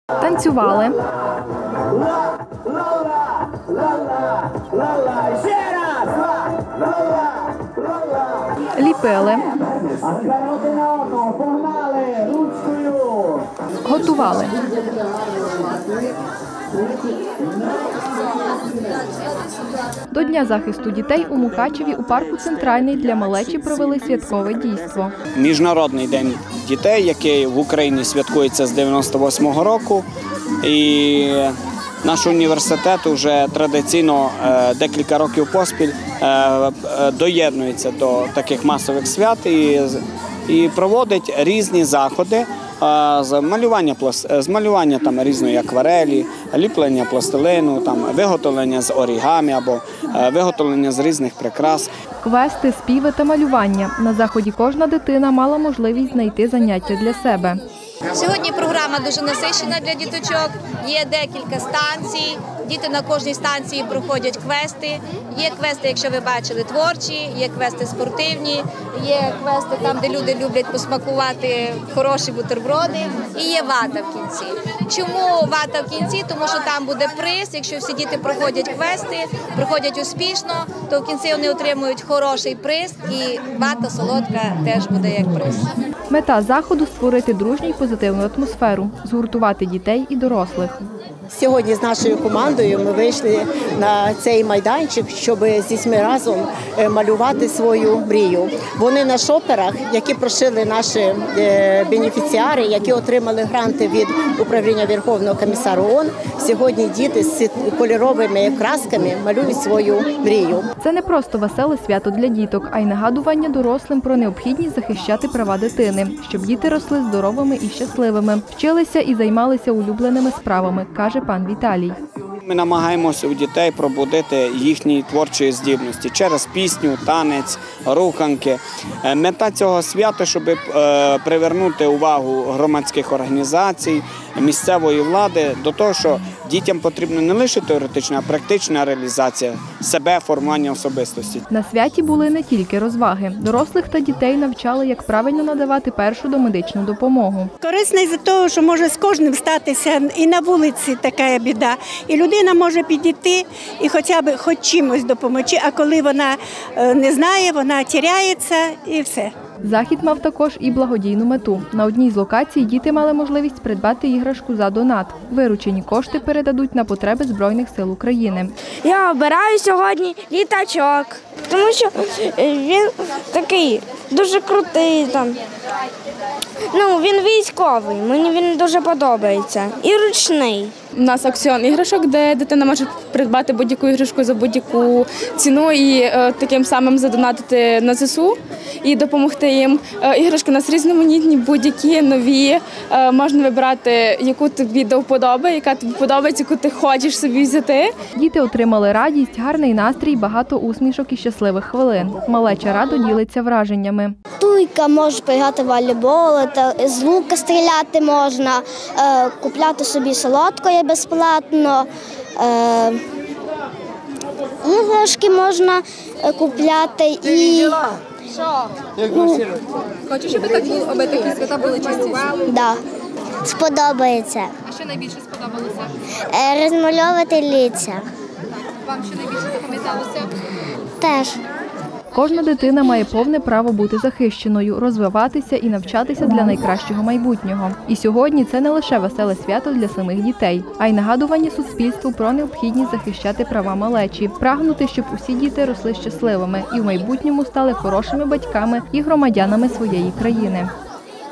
Свято до Дня захисту дітей
Уже традиційно 1 червня відзначається Міжнародний день захисту дітей ‒ прекрасне свято радості та надії. Напередодні для малечі у Мукачеві провели святкове дійство. Квести, співи та малювання на заході кожна дитина мала можливість знайти заняття для себе. Мета дійства створити дружню й позитивну атмосферу, згуртувати дітей і дорослих. Це не просто веселе свято для діток, а й нагадування  дорослим про необхідність захищати права дитини , щоб діти росли  здоровими і щасливими, вчилися і займалися улюбленими справами, каже пан Віталій. На святі були не тільки розваги. Дорослих та дітей навчали як правильно надавати першу домедичну допомогу. Захід мав також і благодійну мету. Виручені кошти передадуть на потреби ЗСУ. Діти отримали радість, гарний настрій, багато усмішок і щасливих хвилин. Малеча радо ділиться враженнями. Кожна дитина має повне право бути захищеною, розвиватися й навчатися для кращого майбутнього. І сьогодні – це не лише веселе свято для самих дітей, а й нагадування суспільству про необхідність захищати права малечі, прагнути, щоб усі діти росли щасливими і в майбутньому стали хорошими батьками і громадянами своєї країни.